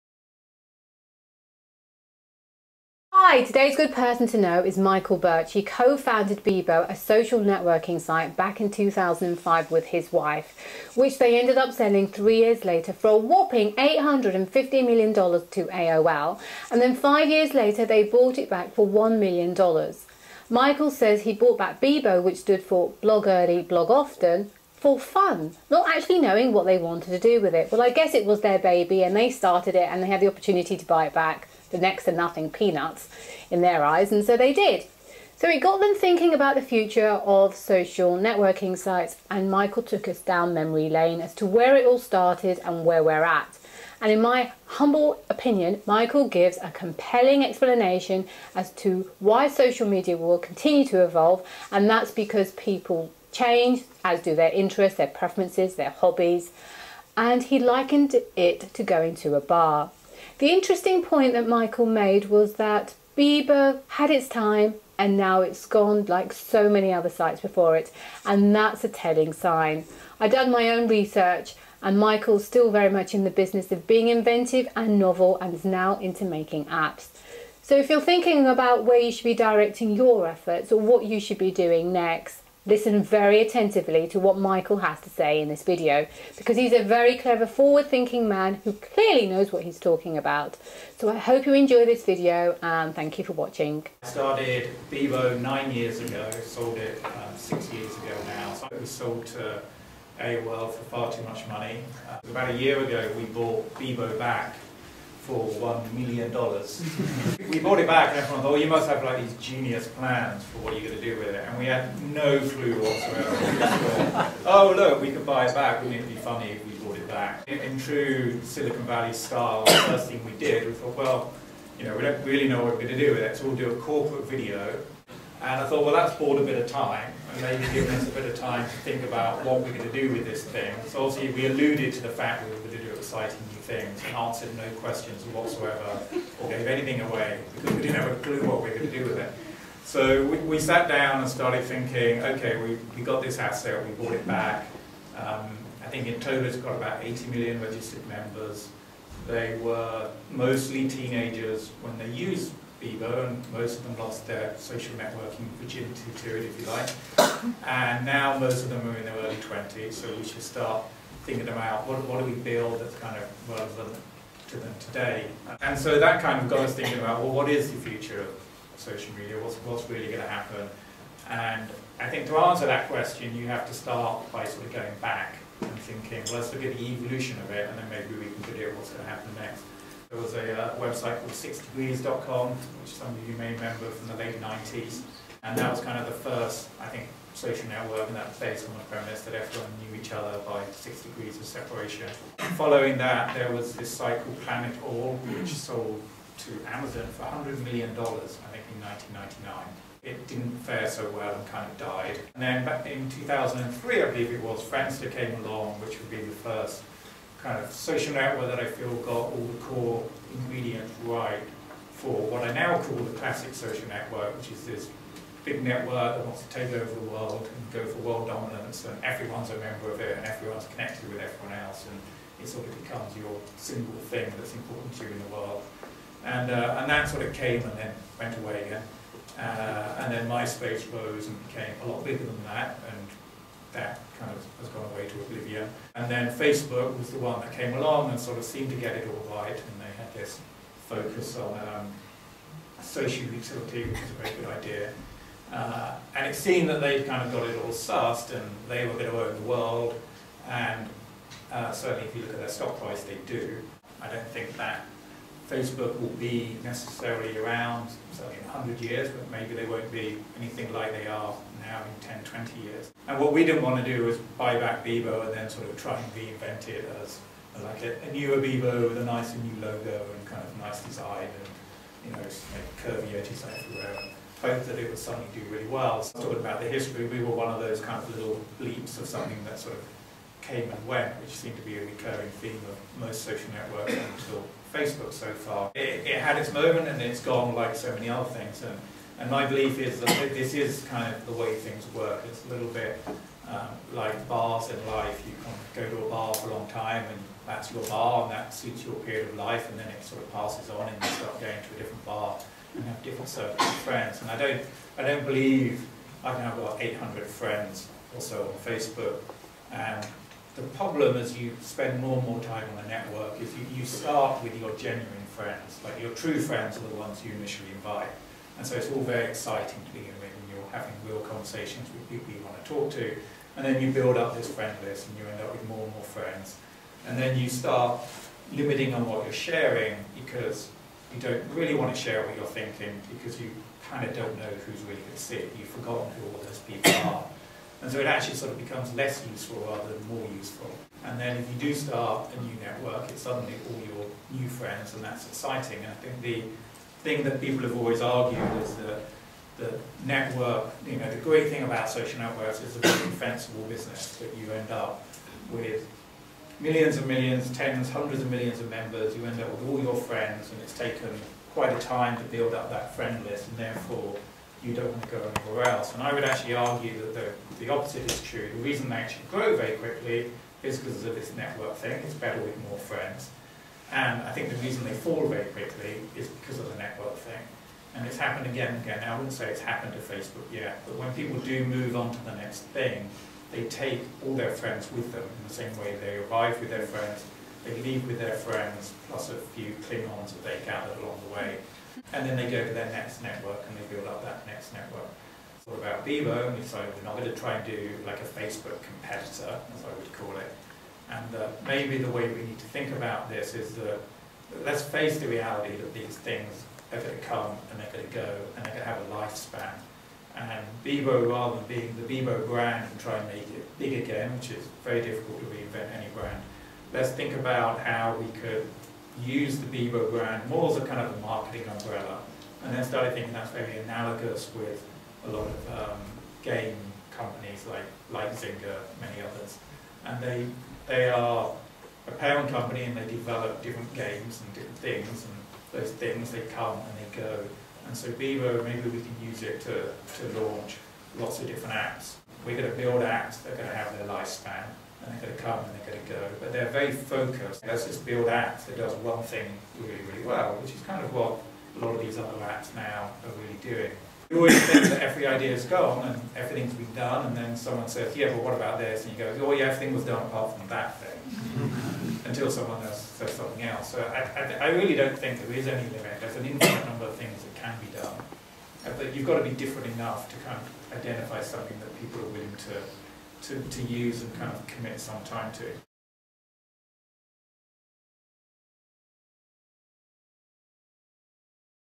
Future of Social Media | GPTK | Michael Birch
Everyones surely heard of Michael Birch, co-founder of the social networking site Bebo which he built with his wife in 2005 and then sold 3 years later to AOL for a staggering $850m.  Unfortunately AOL ran it to the ground so they bought it back for $1m just because they could... fun... it was their baby!  In this talk Michael goes down memory lane and because history has a habit of repeating itself, Michael said Bebo had its time, that social networking sites will continue to evolve and likened it to bars. Place to go, chill out, have a laugh until the next trendy bar comes along and then that becomes the hottest place to go.  Michael obviously knows what he's talking about so listen very carefully and maybe you'll be able to develop something similar to the success of Bebo for the next generation...